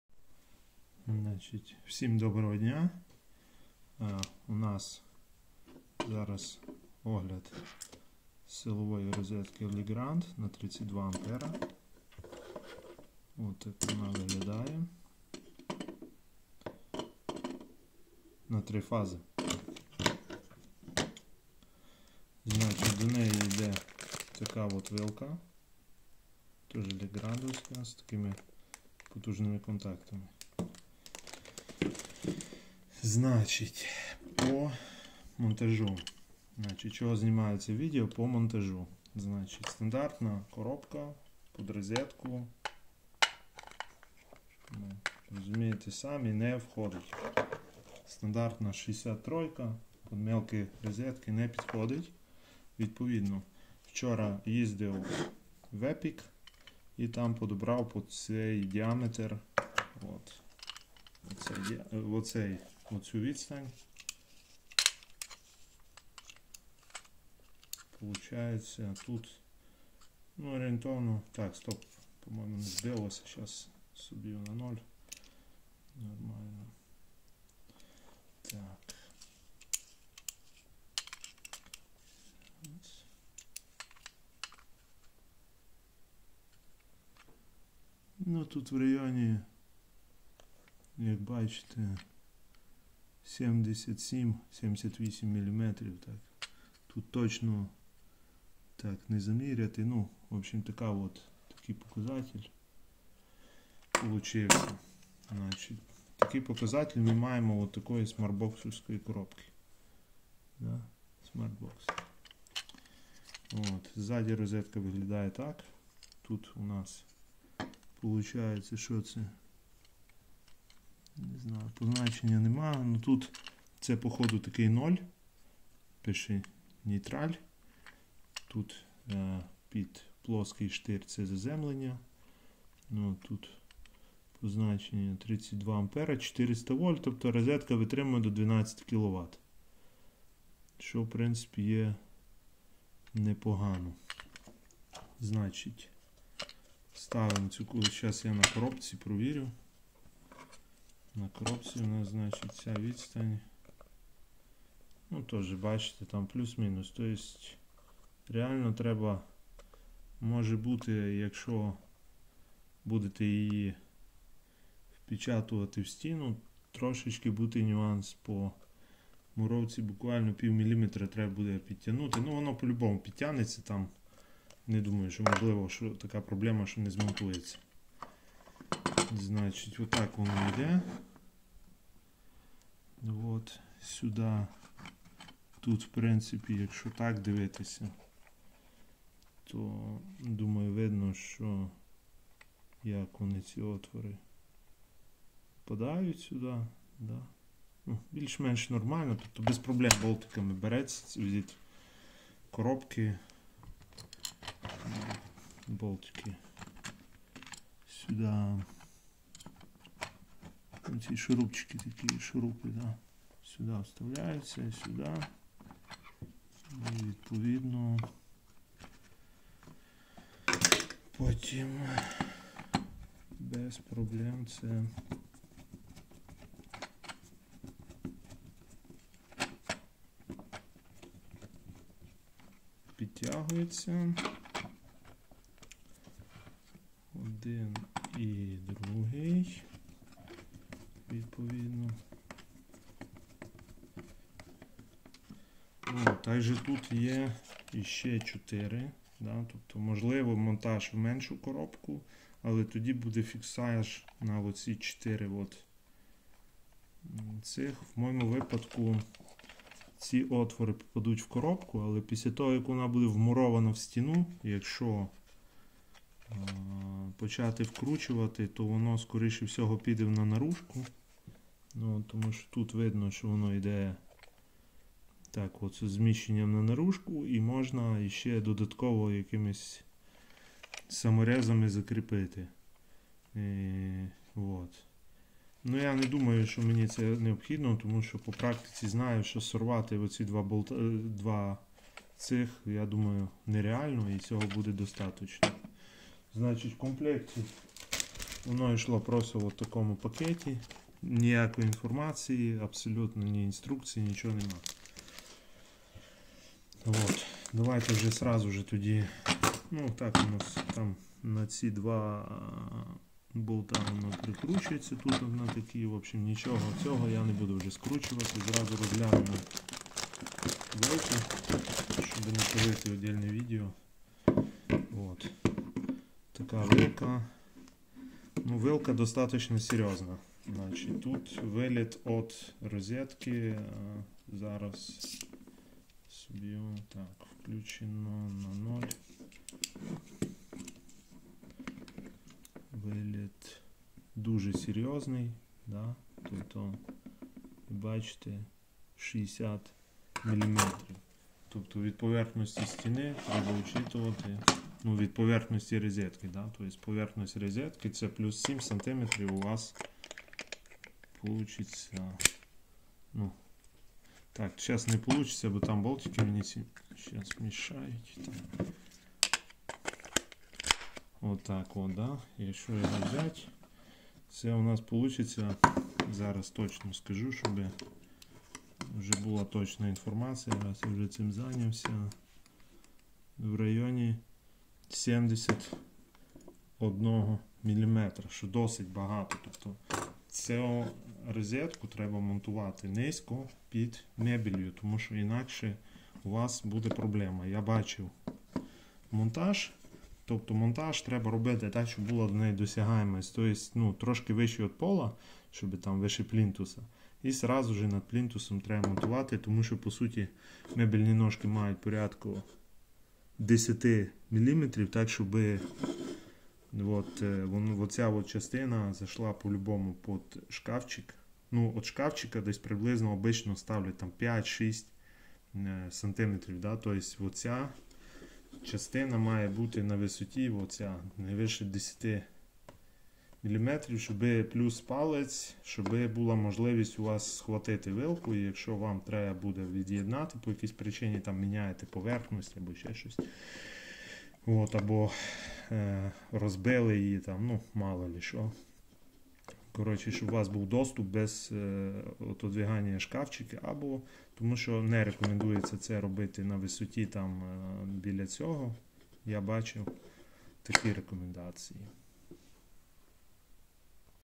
Розетка Legrand 3х фазна 32А
У використанні з плитою Hansa на 2-х фазах розетка чудово себе показує, жодних проблем немає. Уже 3 місяці пройшло.